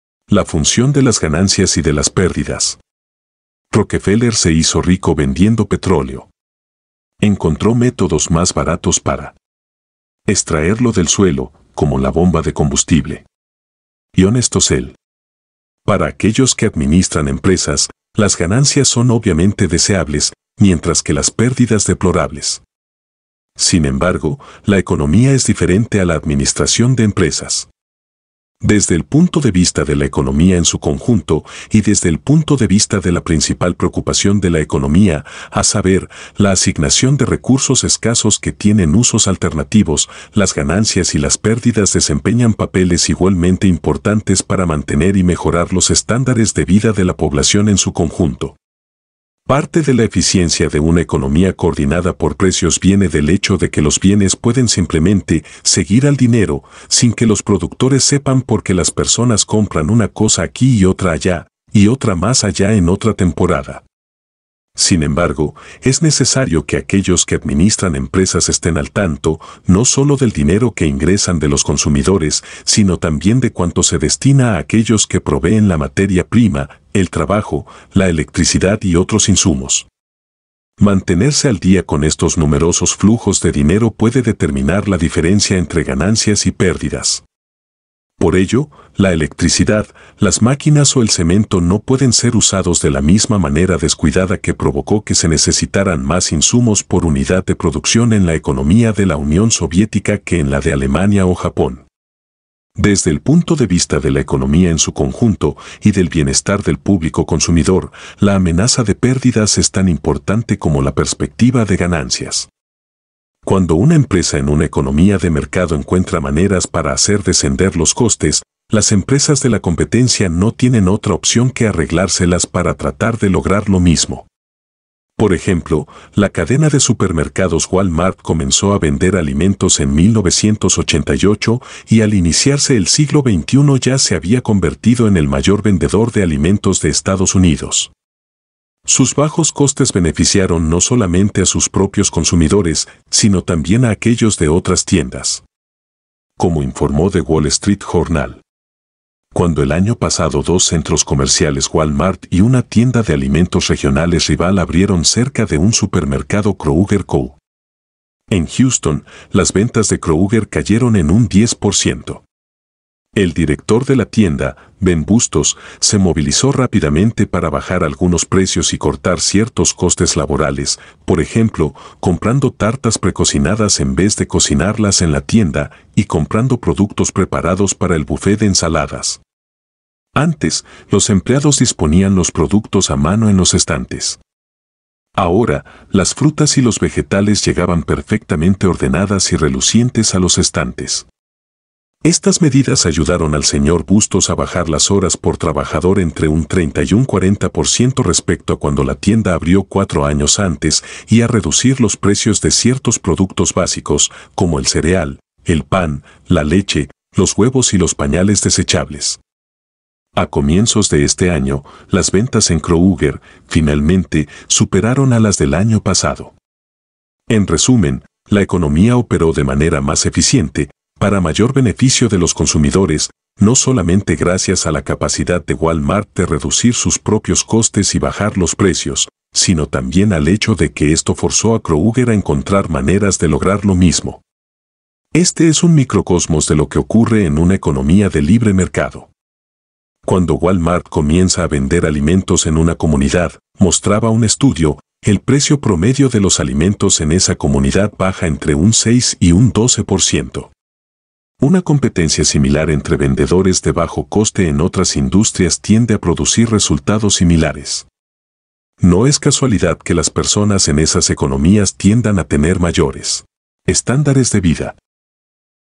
Thomas Sowell - La función de las ganancias y de las pérdidas
LA FUNCIÓN DE LAS GANANCIAS Y DE LAS PÉRDIDAS

Rockefeller se hizo rico vendiendo petróleo... Encontró métodos más baratos para
extraerlo del suelo, como la bomba de combustible.
JOHN STOSSEL

Para aquellos que administran empresas, las ganancias son obviamente deseables, mientras que las pérdidas deplorables. Sin embargo, la economía es diferente a la administración de empresas. Desde el punto de vista de la economía en su conjunto, y desde el punto de vista de la principal preocupación de la economía —a saber, la asignación de recursos escasos que tienen usos alternativos— las ganancias y las pérdidas desempeñan papeles igualmente importantes para mantener y mejorar los estándares de vida de la población en su conjunto.

Parte de la eficiencia de una economía coordinada por precios viene del hecho de que los bienes pueden simplemente «seguir al dinero», sin que los productores sepan por qué las personas compran una cosa aquí y otra allá, y otra más allá en otra temporada. Sin embargo, es necesario que aquellos que administran empresas estén al tanto, no sólo del dinero que ingresan de los consumidores, sino también de cuánto se destina a aquellos que proveen la materia prima, el trabajo, la electricidad y otros insumos. Mantenerse al día con estos numerosos flujos de dinero puede determinar la diferencia entre ganancias y pérdidas. Por ello, la electricidad, las máquinas o el cemento no pueden ser usados de la misma manera descuidada que provocó que se necesitaran más insumos por unidad de producción en la economía de la Unión Soviética que en la de Alemania o Japón. Desde el punto de vista de la economía en su conjunto, y del bienestar del público consumidor, la amenaza de pérdidas es tan importante como la perspectiva de ganancias. Cuando una empresa en una economía de mercado encuentra maneras para hacer descender los costes, las empresas de la competencia no tienen otra opción que arreglárselas para tratar de lograr lo mismo.

Por ejemplo, la cadena de supermercados Wal-Mart comenzó a vender alimentos en 1988, y al iniciarse el siglo XXI ya se había convertido en el mayor vendedor de alimentos de Estados Unidos. Sus bajos costes beneficiaron no solamente a sus propios consumidores, sino también a aquellos de otras tiendas. Como informó The Wall Street Journal:

Cuando el año pasado dos centros comerciales Wal-Mart y una tienda de alimentos regionales rival abrieron cerca de un supermercado Kroger Co. En Houston, las ventas de Kroger cayeron en un 10 por ciento. El director de la tienda, Ben Bustos, se movilizó rápidamente para bajar algunos precios y cortar ciertos costes laborales, por ejemplo, comprando tartas precocinadas en vez de cocinarlas en la tienda, y comprando productos preparados para el bufé de ensaladas. Antes, los empleados disponían los productos a mano en los estantes.

Ahora, las frutas y los vegetales llegaban perfectamente ordenadas y relucientes a los estantes.
Estas medidas ayudaron al señor Bustos a bajar las horas por trabajador entre un 30 y un 40 por ciento respecto a cuando la tienda abrió cuatro años antes, y a reducir los precios de ciertos productos básicos, como el cereal, el pan, la leche, los huevos y los pañales desechables. A comienzos de este año, las ventas en Kroger, finalmente, superaron a las del año pasado.